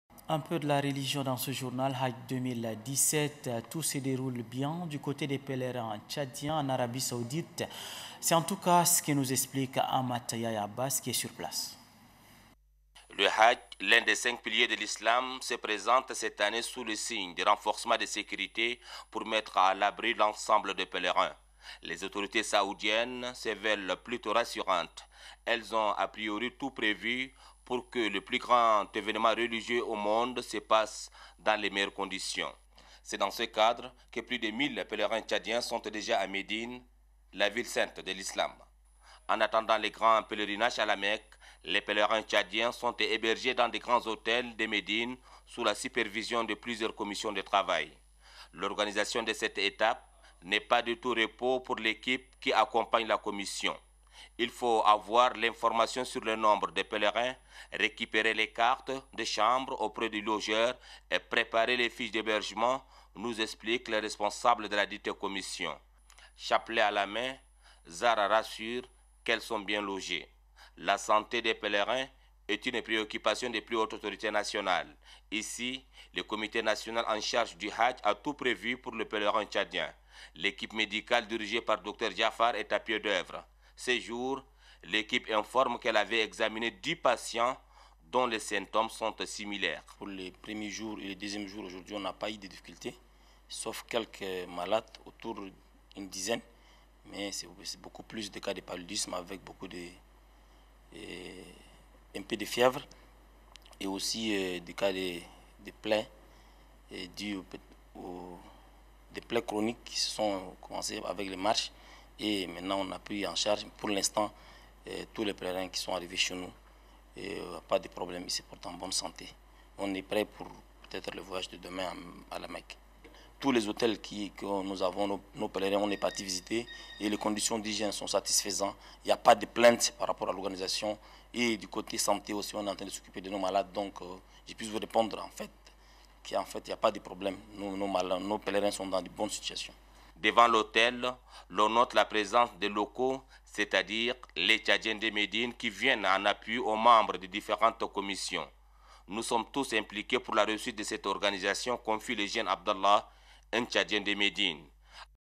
INFOS TELE TCHAD DU 21 AOUT 2017 - Hadj 2017
Titre
1) Sejour des pelérins Tchadiens à Médine